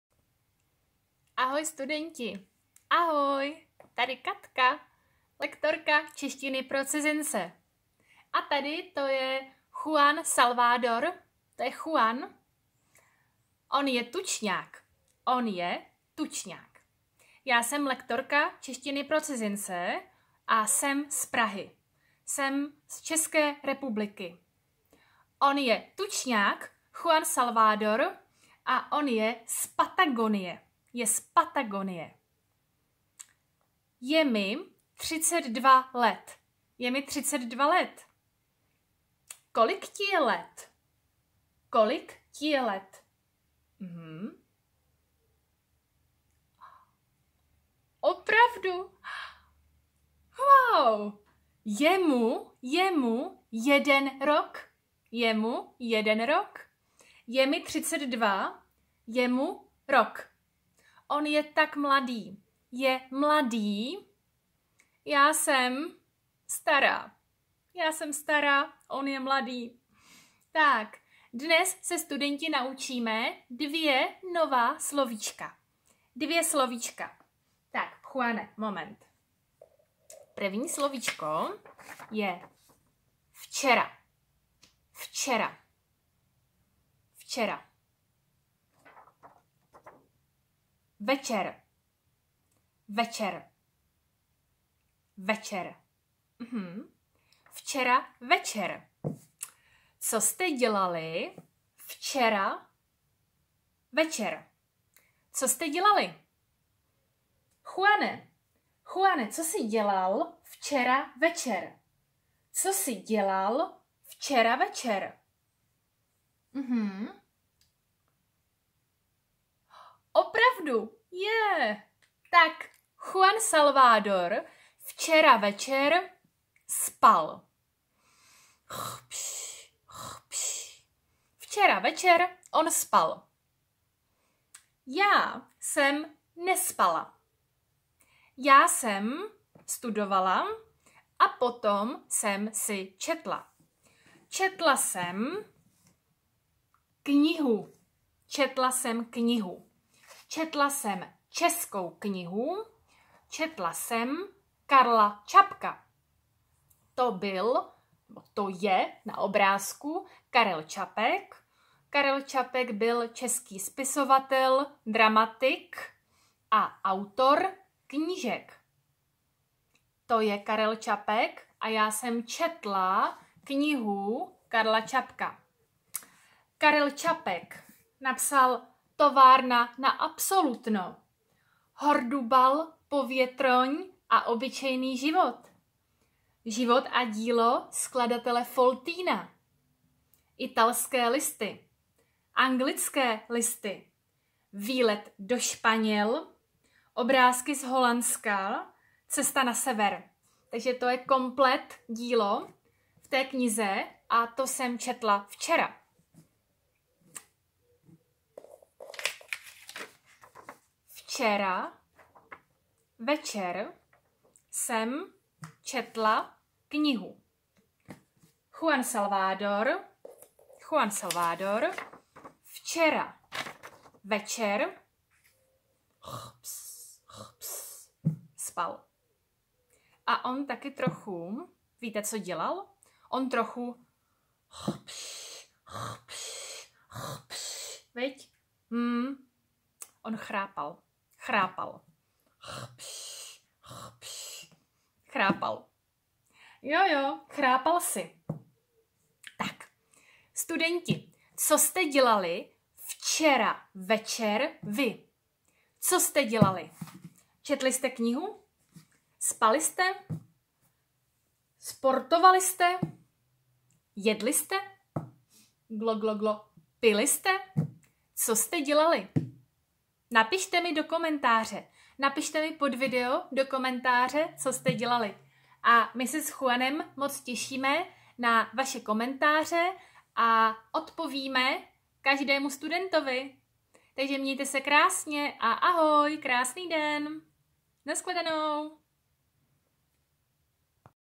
Co jste dělali včera večer_čeština pro cizince_A1/A2
Another video about Czech in Czech and with Czech subtitles. Learn Czech with Katka!
This video has subtitles. Turn them on!

Ahoj studenti, taky Katka - lektorka češtiny pro cizince. Jsem z Prahy, z České republiky. A to je Juan Salvador - tučňák. Je z Patagonie.
Chcete se s námi učit češtinu online?

Dnešní téma je: CO JSTE DĚLALI VČERA VEČER?
Podívejte se na video.
1. Co dělala včera večer Katka?
2. Co dělal včera večer Juan Salvador?

A teď to důležité: Co jste dělali včera večer vy?

Napište nám pod video do komentáře!
Těšíme se!
Katka + Juan